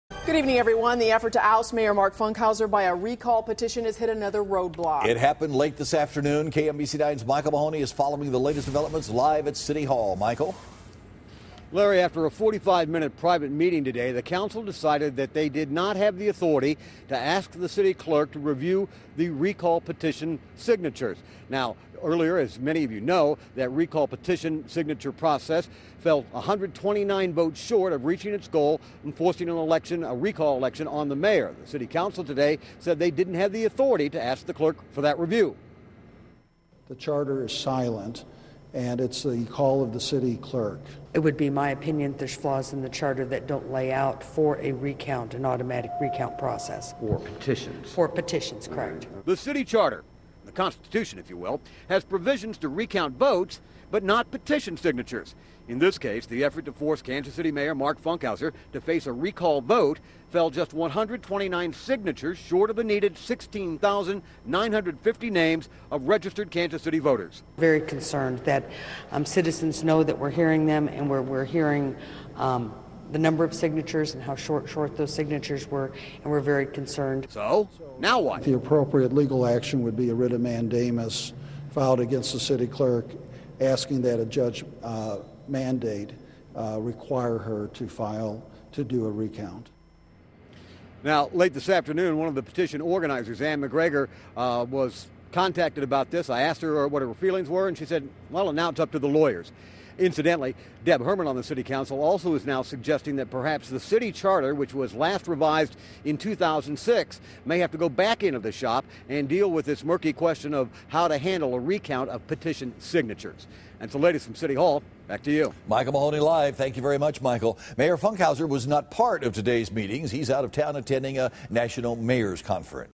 Council Won't Ask Clerk To Review Recall Signatures
The City Council said on Thursday it will not ask the City Clerk to review the recall petition against Kansas City Mayor Mark Funkhouser.